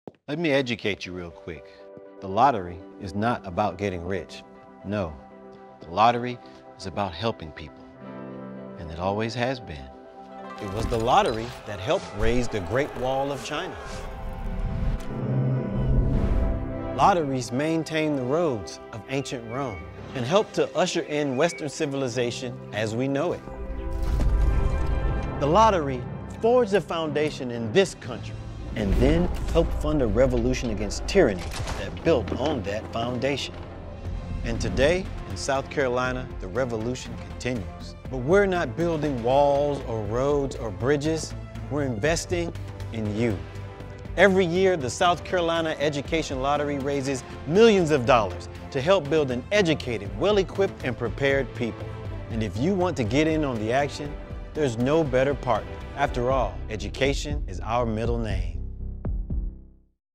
Better U
Introducing the Better U program from the South Carolina Education Lottery, a new education campaign that hopes to educate the public on odds, problem gambling symptoms, and common myths about the Lottery.